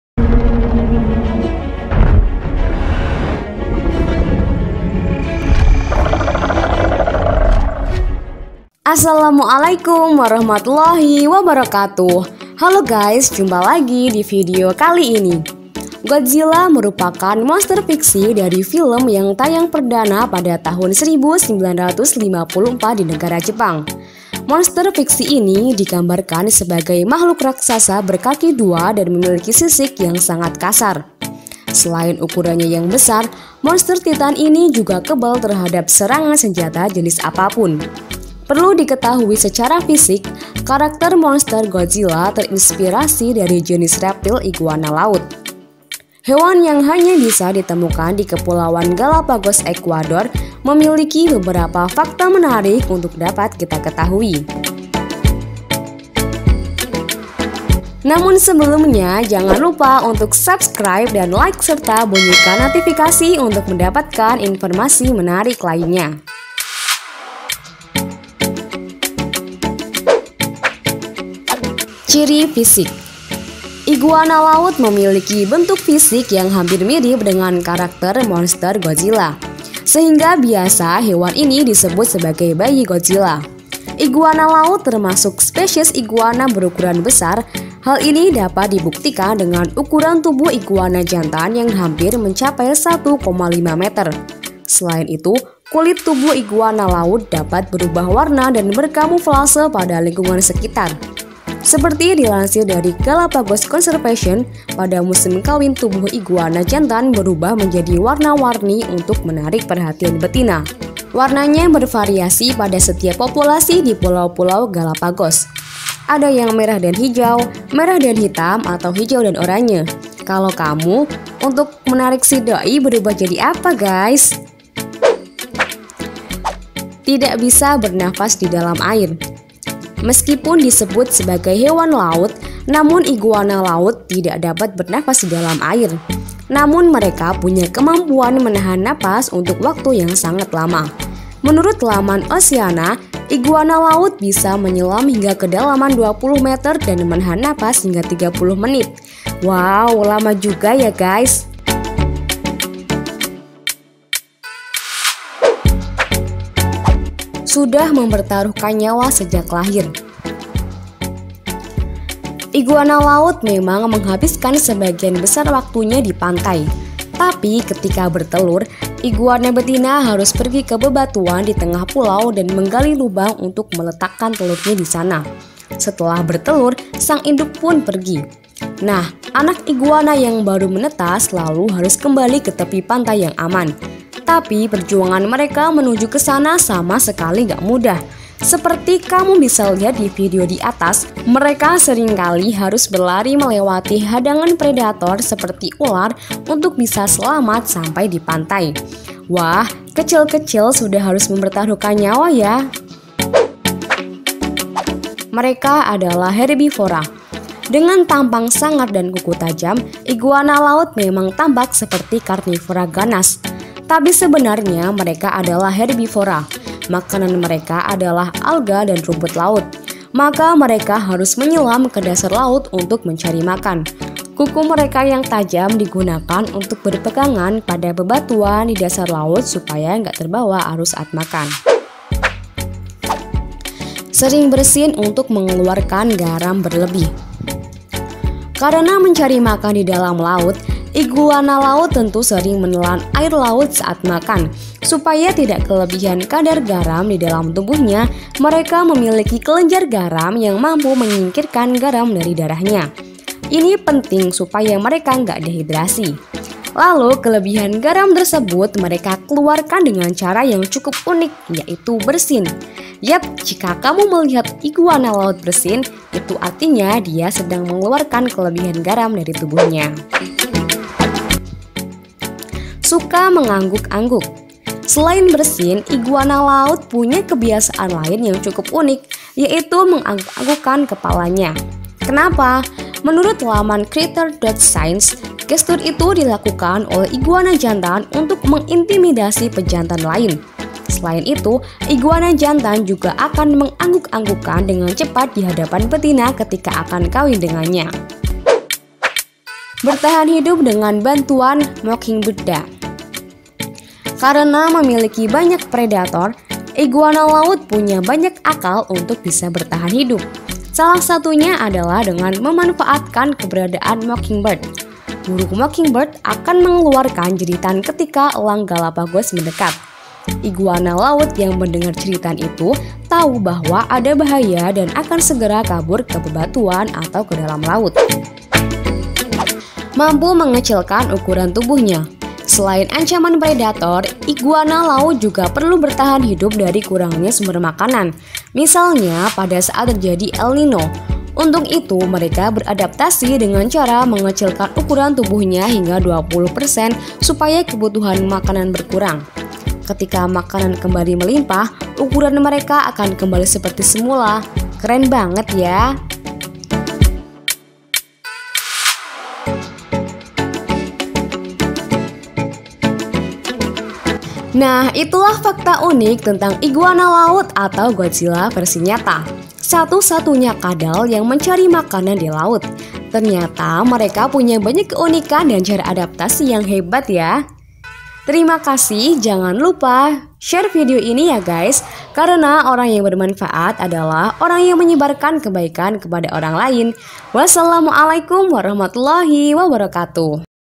Menggegerkan 'Godzilla' asli dunia nyata inilah FAKTA Bayi Godzilla
Godzilla merupakan monster fiksi dari film yang tayang perdana pada tahun 1954 di negara Jepang. Monster fiksi ini digambarkan sebagi mahkluk raksasa berkaki dua dan memiliki sisik yang sangat kasar, selain ukurannya yang besar, monster titan ini juga kebal terhadap serangan senjata jenis apapun. Perlu diketahui secara fisik, karakter monster godzila terinspirasi dari jenis reptil iguana laut, atau yang memiliki nama latin Amblyrhynchus cristatus. Hewan yang hanya bisa ditemukan di kepulauan Galapagos-Equador, memiliki beberapa fakta menarik untuk dapat kita ketahui.

Jika ada Keperluan dengan kami
Silahkan email kami di: